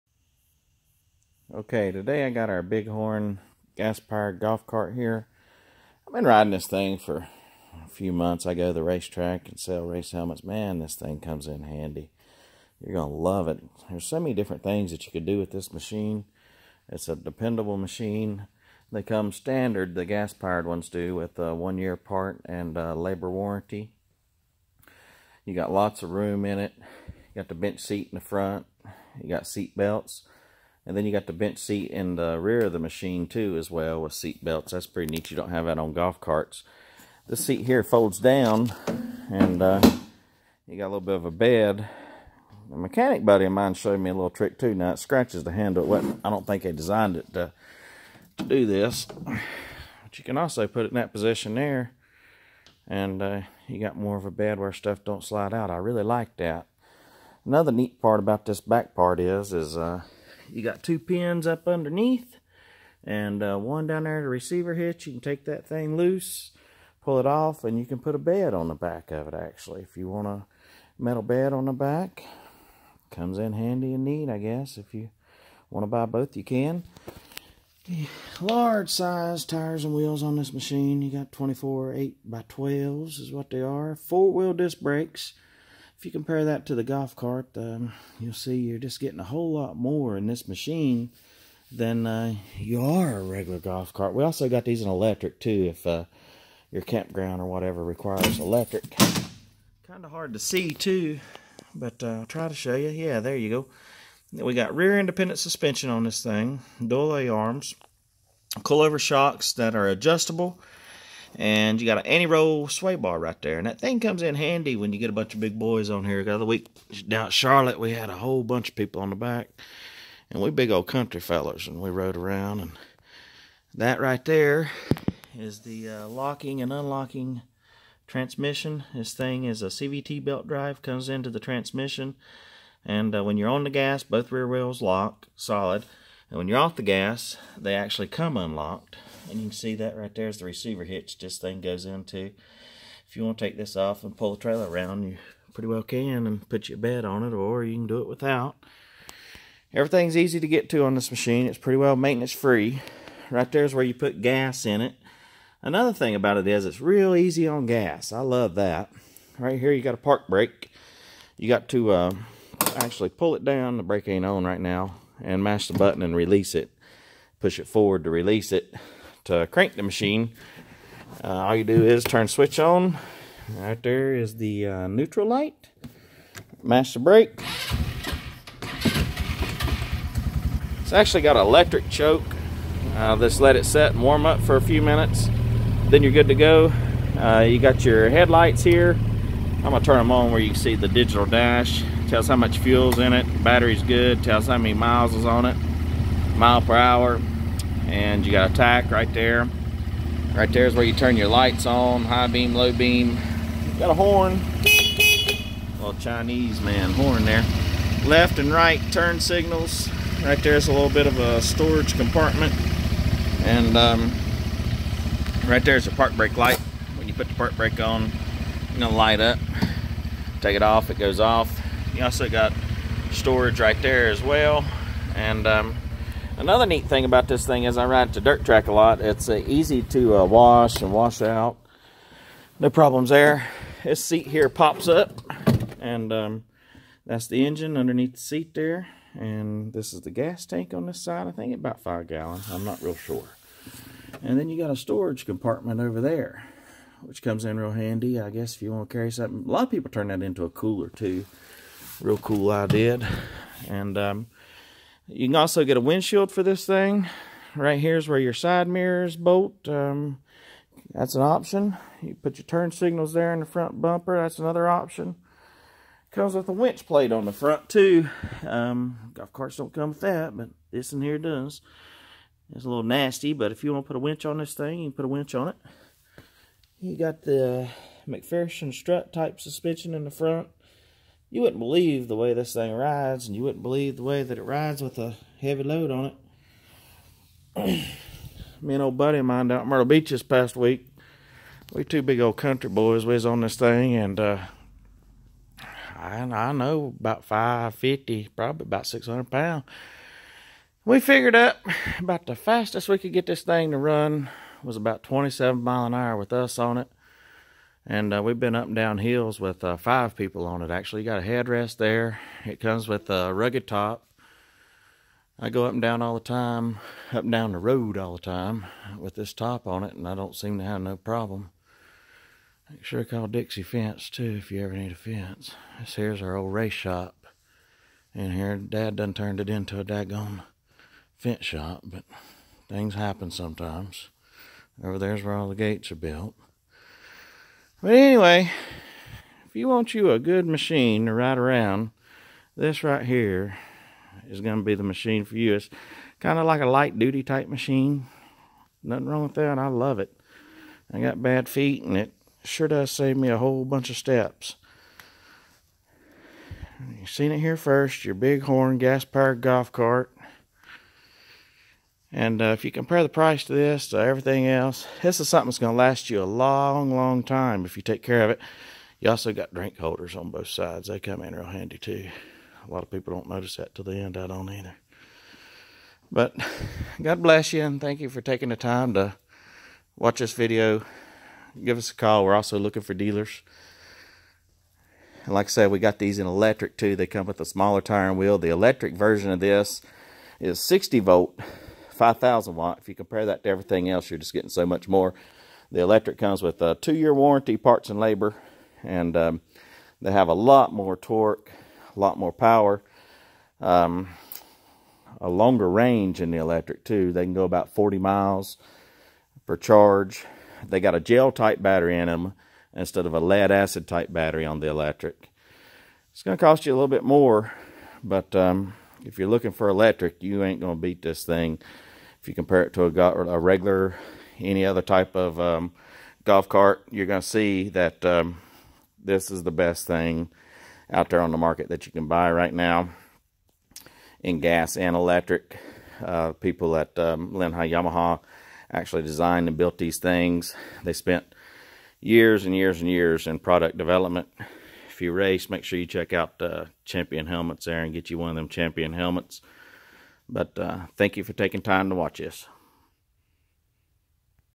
Bighorn Gas Golf Cart
What a all some gas golf cart. Compare to easy go and club car golf carts. You will see this machine is built totally different. 4 wheel disk Brakes. Independent suspension and so much more.great to use around the house or camp ground.  Designed by the people at linhai yamaha.